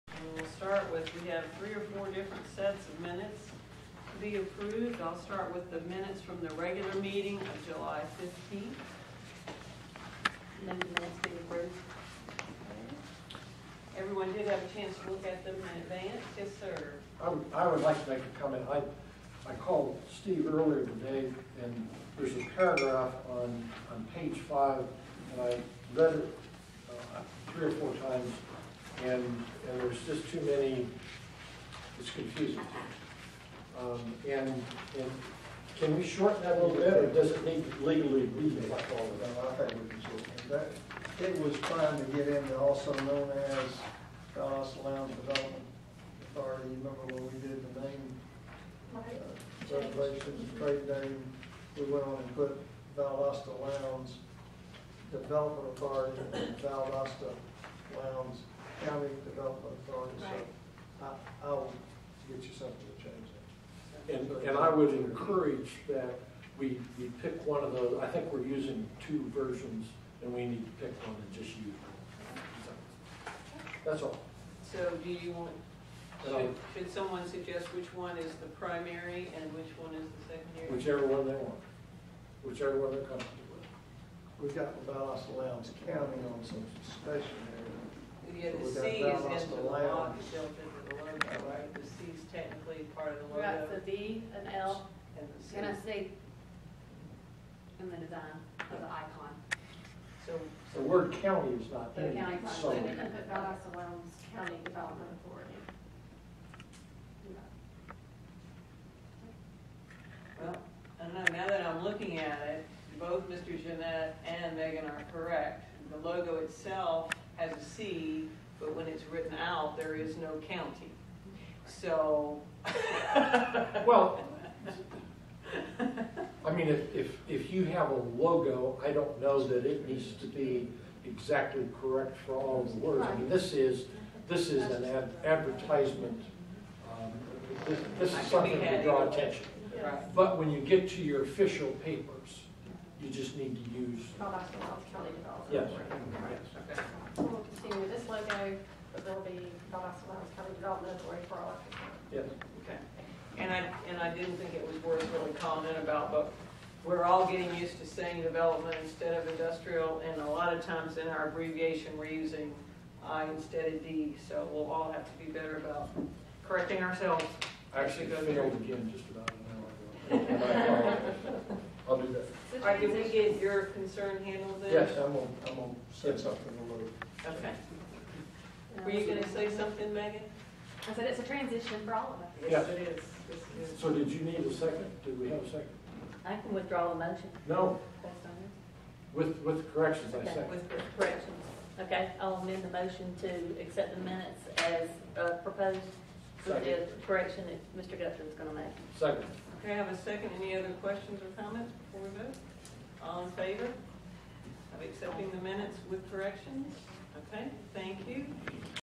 Minutes @ VLCIA 2014-08-19
egular Meeting, Valdosta-Lowndes County Industrial Authority (VLCIA)
Tom Call, Roy Copeland, Mary Gooding, Chairman, Jerry Jennett, Terri Lupo